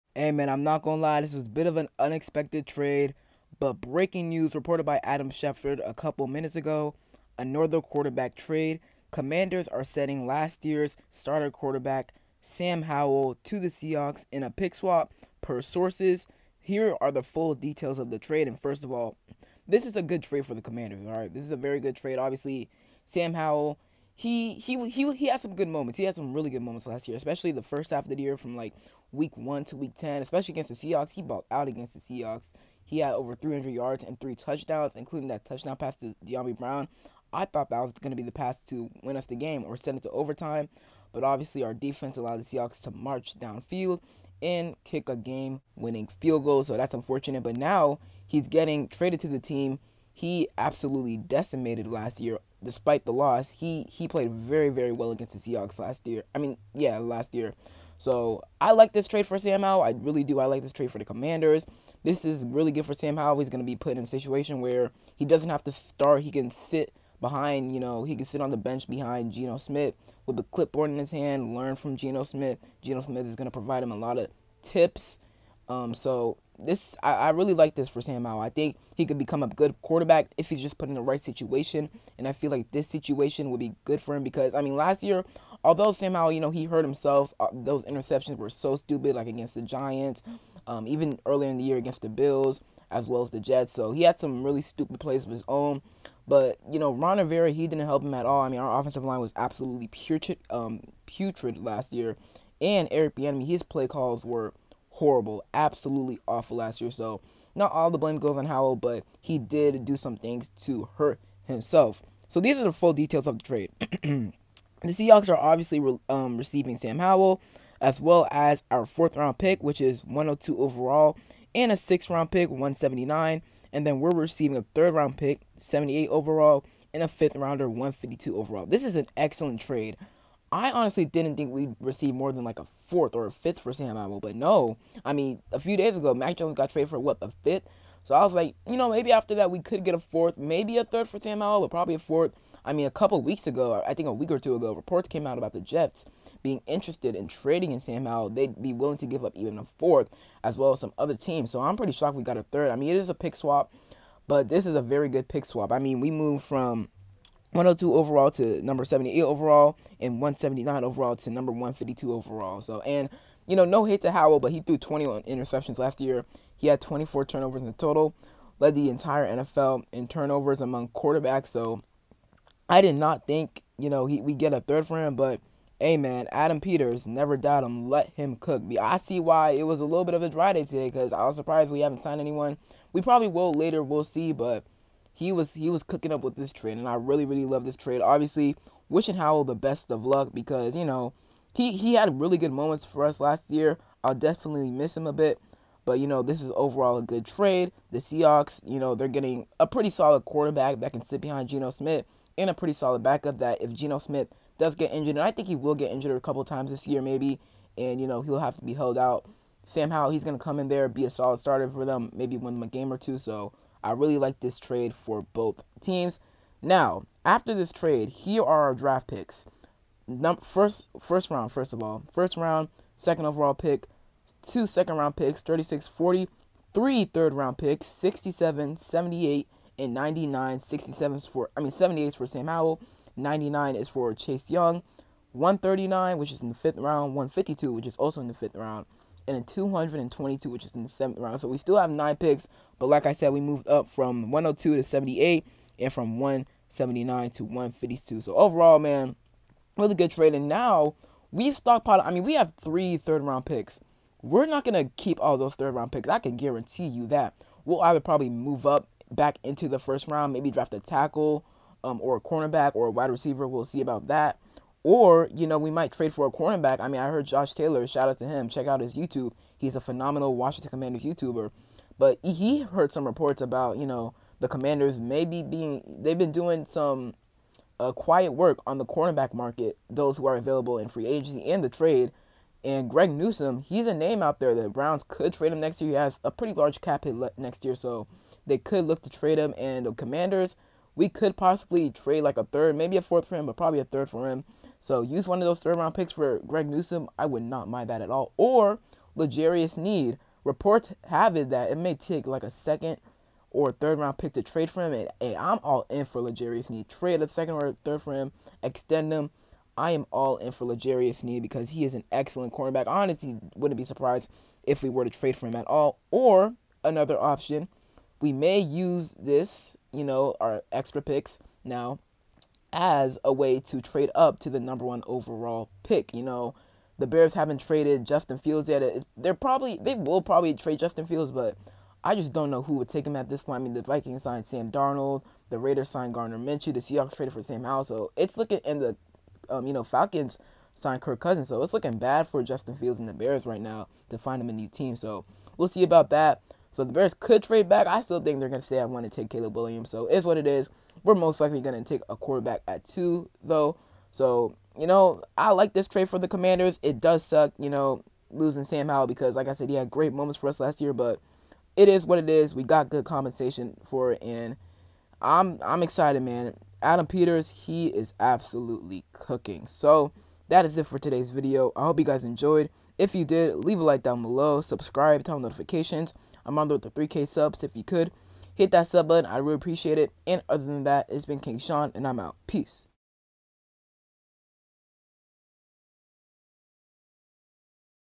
🚨 Commanders Trade QB Sam Howell to the Seattle Seahawks
Washington Commanders are sending Sam Howell to the Seahawks.